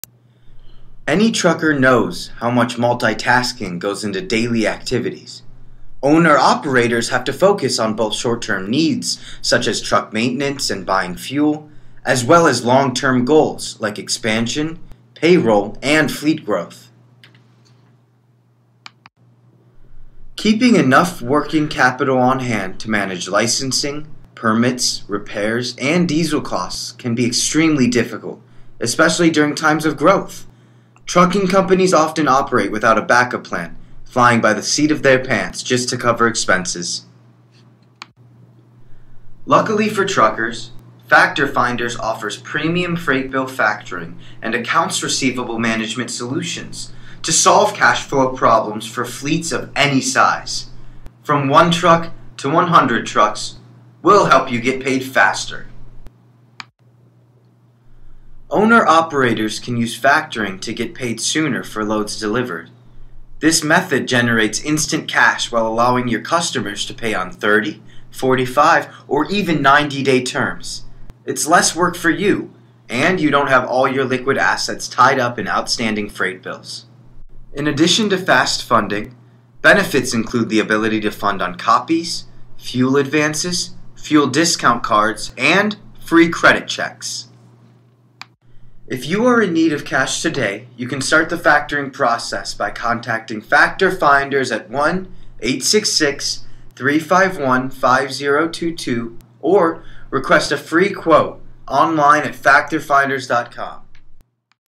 Factoring for Truckers
Any trucker knows how much multitasking goes into daily activities. Owner-operators have to focus on both short term needs, such as truck maintenance and buying fuel, as well as long term goals like expansion, payroll and fleet growth.

Keeping enough working capital on hand to manage licensing, permits, repairs, and diesel costs can be extremely difficult, especially during times of growth. Trucking companies often operate without a backup plan, flying by the seat of their pants just to cover expenses.

Luckily for truckers, Factor Finders offers premium freight bill factoring and accounts receivable management solutions to solve cash flow problems for fleets of any size. From one truck to one hundred trucks, we’ll help you get paid faster.